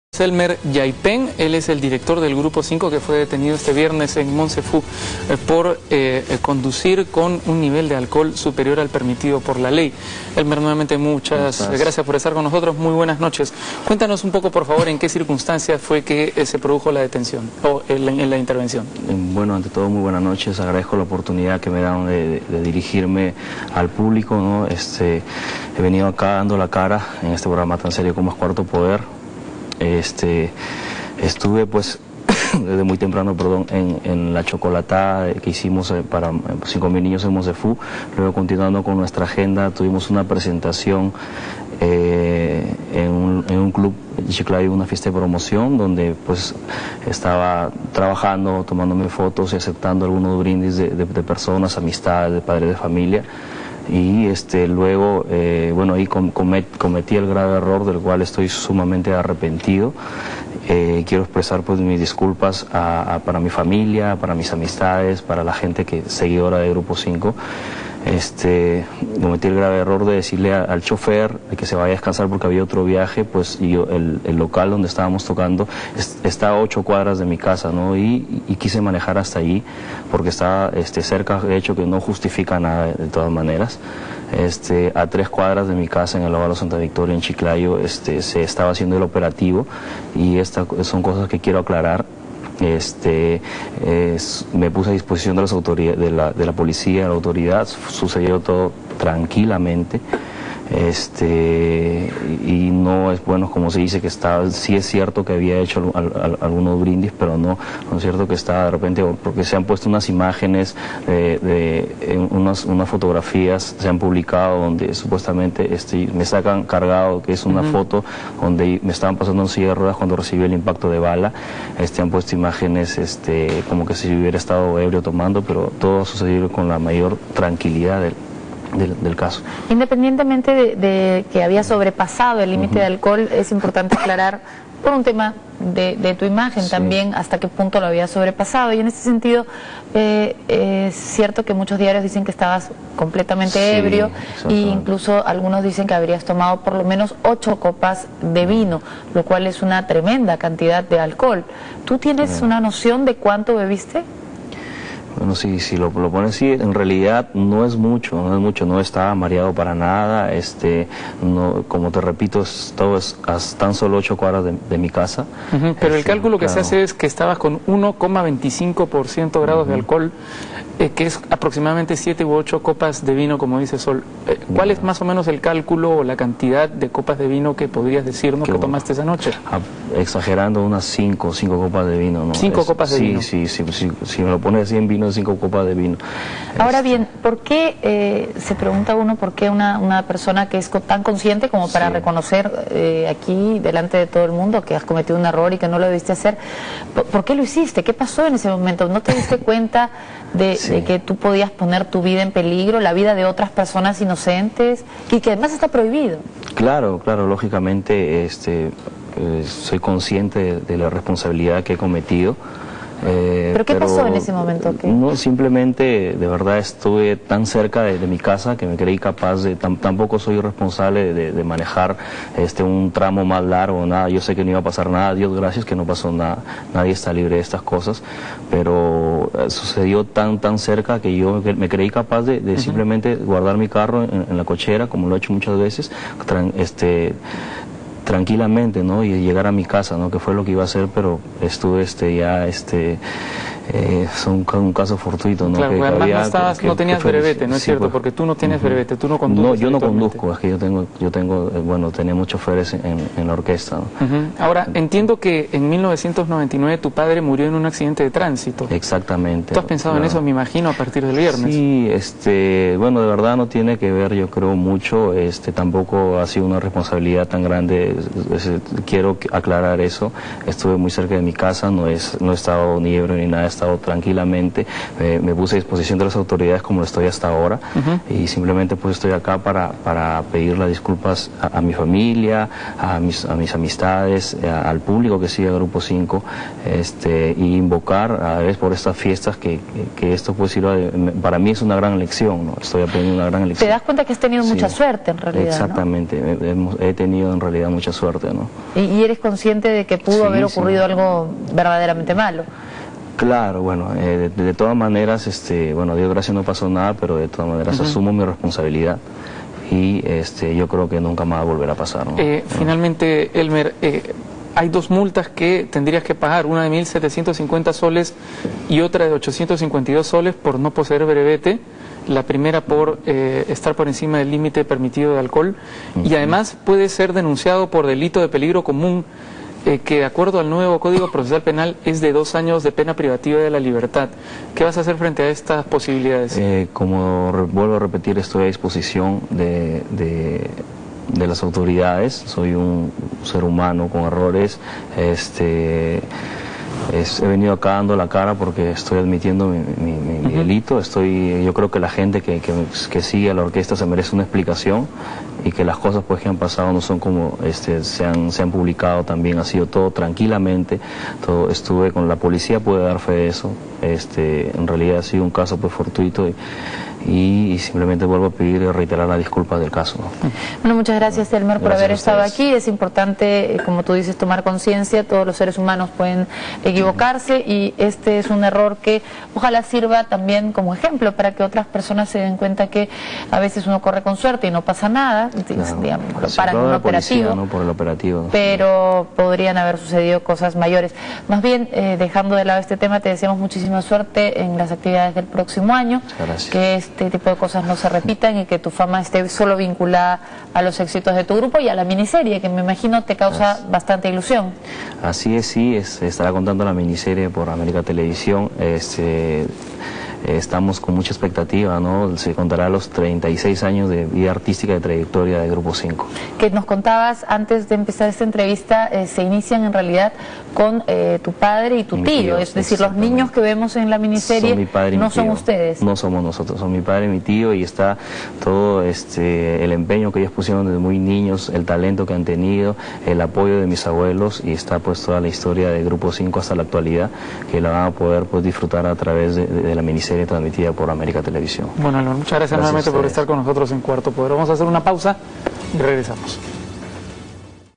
Grupo 5: Elmer Yaipen pide disculpas por manejar con 1.25 de alcohol
20 Diciembre 2009. 'Cometí el grave error de decirle al chofer que se vaya a descansar porque al siguiente día teníamos que viajar y decidí manejar', dijo el líder del Grupo 5. Fuente: América TV.